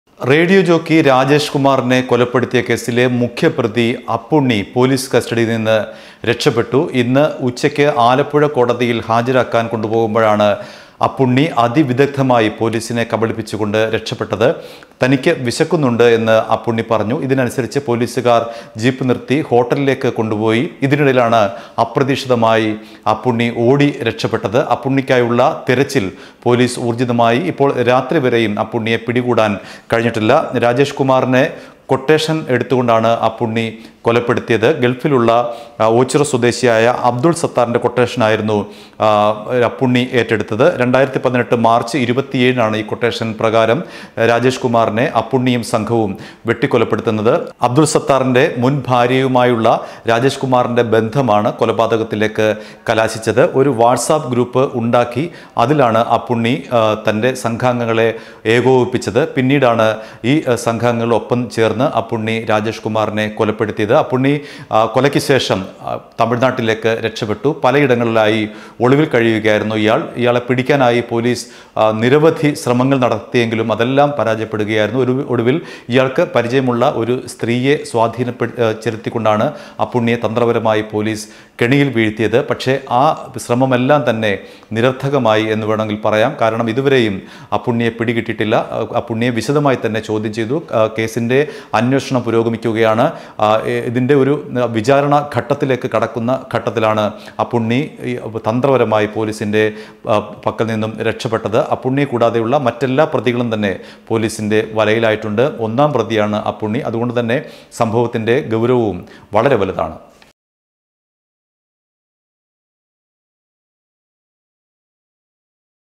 രാജേഷ് വധക്കേസ് പ്രതി അപ്പുണ്ണി കസ്റ്റഡിയില്‍ നിന്നും രക്ഷപെട്ടു I Radio Jockey Rajesh
റേഡിയോ ജോക്കി രാജേഷിനെ കൊലപ്പെടുത്തിയ കേസിലെ പ്രതി അപ്പുണ്ണി പോലീസ് കസ്റ്റഡിയില്‍ നിന്നും രക്ഷപെട്ടു. ആലപ്പുഴ കോടതിയില്‍ ഹാജരാക്കാന്‍ കൊണ്ടുപോകുന്നതിനിടെ ഹോട്ടലിലേക്ക് കൊണ്ടുപോകും വഴിയാണ് പ്രതി രക്ഷപെട്ടത്